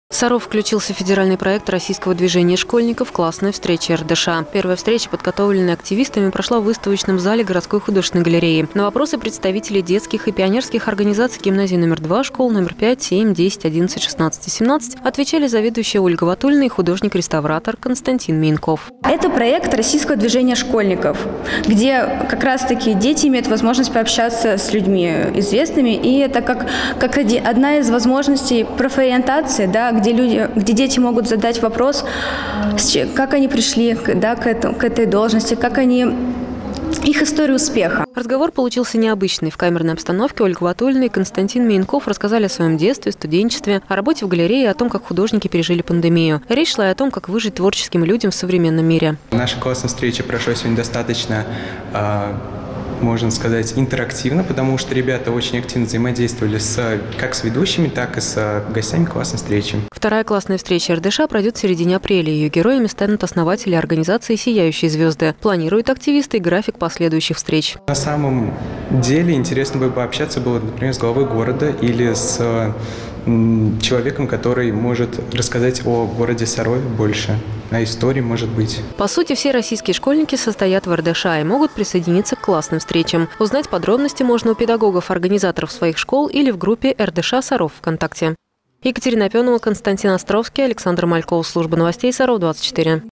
Классная встреча РДШ
Первая «Классная встреча РДШ» прошла в Сарове. Активисты Российского движения школьников посетили Художественную галерею, где их ждали её заведующая, искусствовед Ольга Ватулина и художник Константин Миенков.